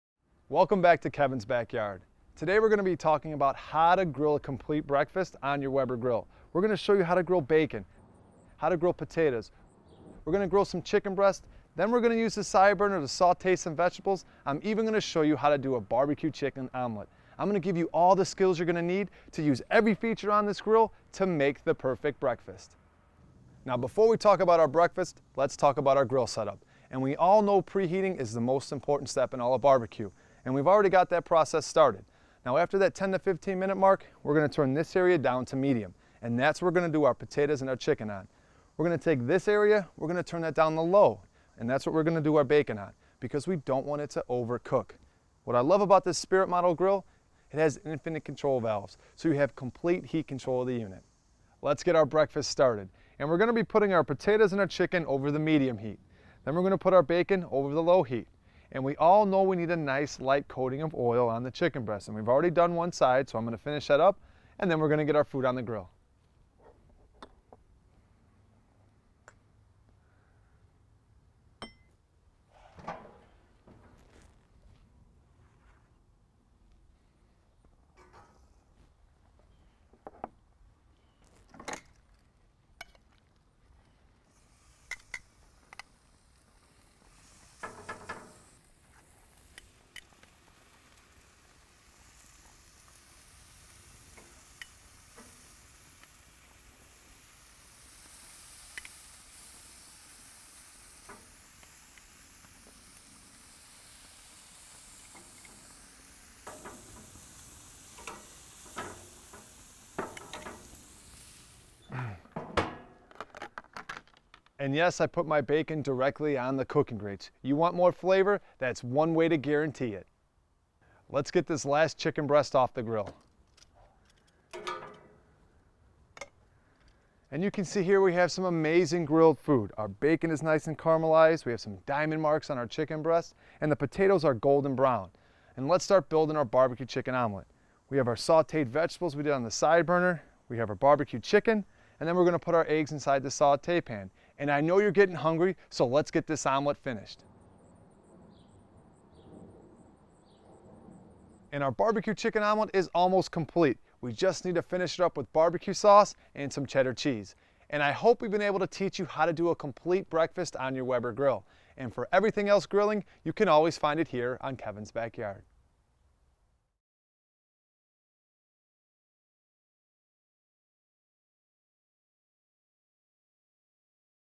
Kevin's Backyard Breaking out of the Box with Breakfast on your Weber!
Breakfast on the grill, the perfect way to start your day.